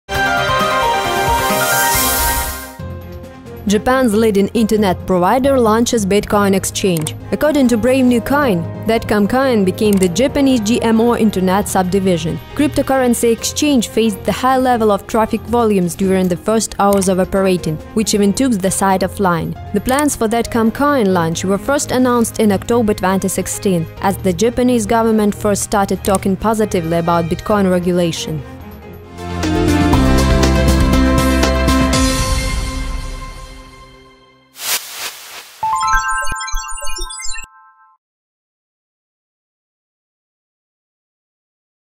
KCN New bitcoin exchange in Japan
Japan's leading Internet-provider launches bitcoin exchange. According to Brave New Coin, Z.com Coin became the Japanese GMO Internet subdivision. Cryptocurrency exchange faced the high levels of traffic volumes during the first hours of operating, which even took the site offline. The plans for Z.com Coin launch were first announced in October 2016, as the Japanese government first started talking positively about Bitcoin regulation.